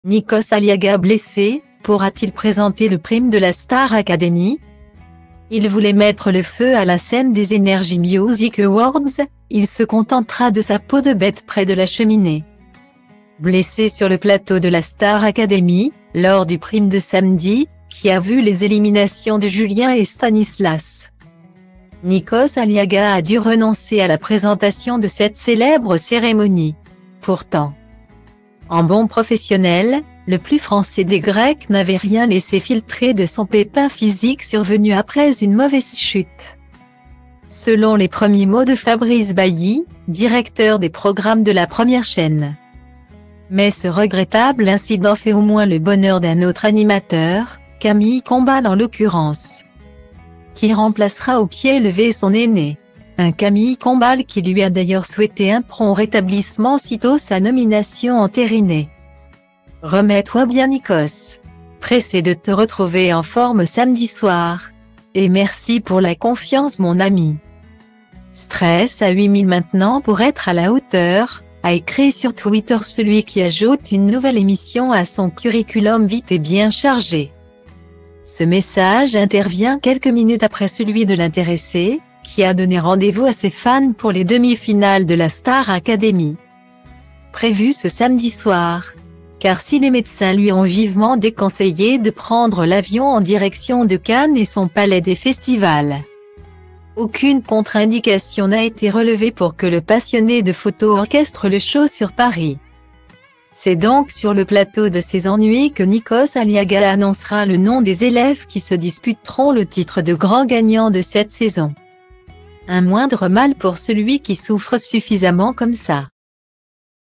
Nikos Aliagas blessé : pourra-t-il présenter le prime de la Star Academy ?
Il voulait mettre le feu à la scène des NRJ Music Awards, il se contentera de sa peau de bête près de la cheminée.Blessé sur le plateau de la Star Academy, lors du prime de samedi, qui a vu les éliminations de Julien et Stanislas,Nikos Aliagas a dû renoncer à la présentation de cette célèbre cérémonie. Pourtant,